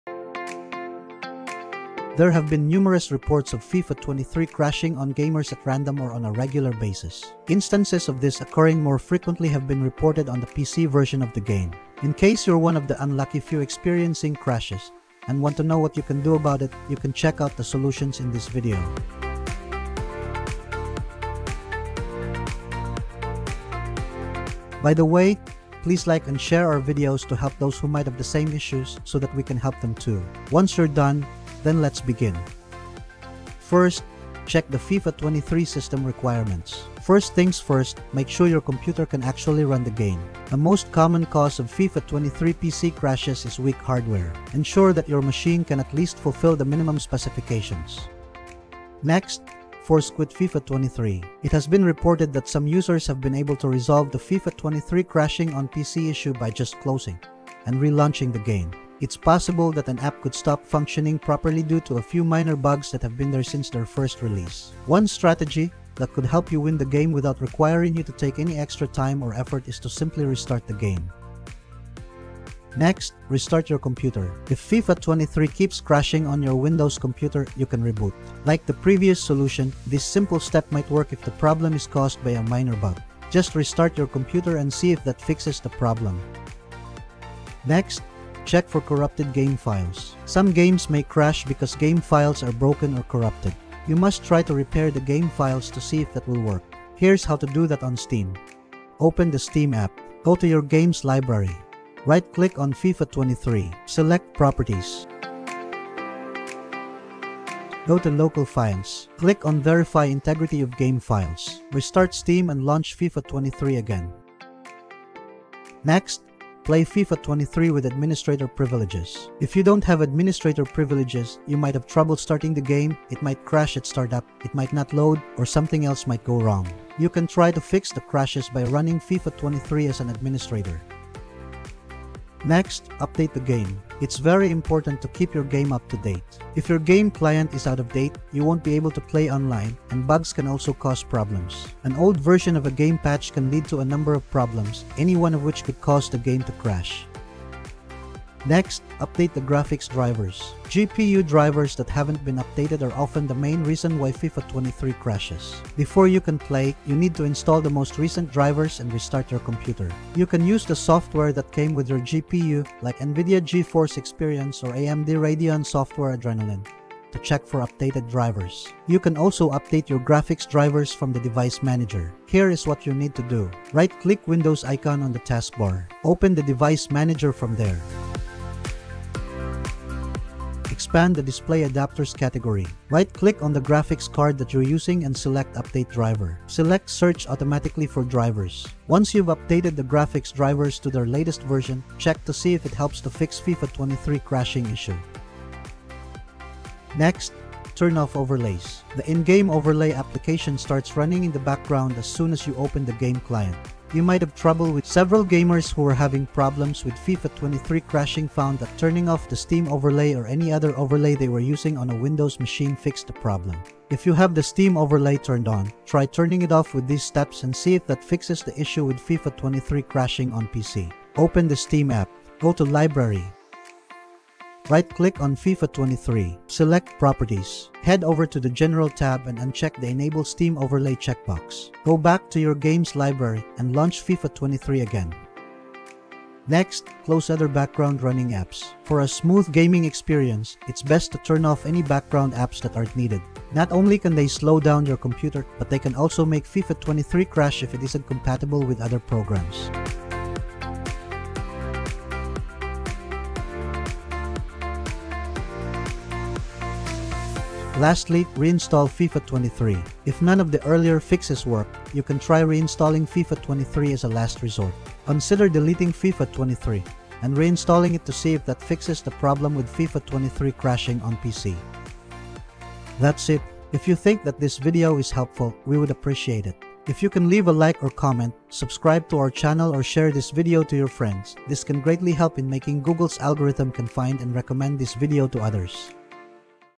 Easy Steps To Fix FIFA 23 Crashing On PC [Updated 2024]
There have been numerous reports of FIFA 23 crashing on gamers at random or on a regular basis. Instances of this occurring more frequently have been reported on the PC version of the game.
In case you're one of the unlucky few experiencing crashes and want to know what you can do about it, you can check out the solutions in this video.